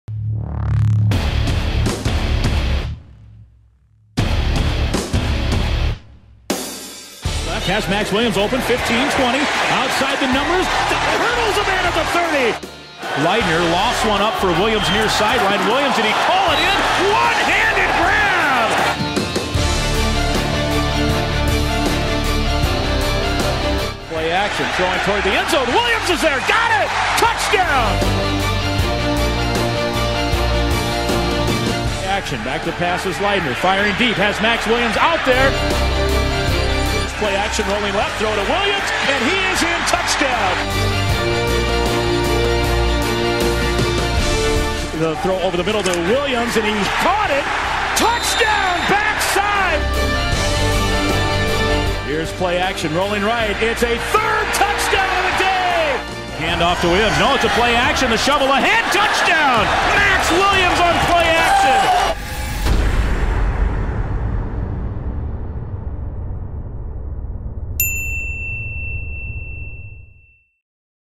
TE Maxx Williams: 2014 Gopher Football Highlights
Watch highlights from Gopher Football tight end Maxx Williams' 2014 season from Gopher Digital Productions!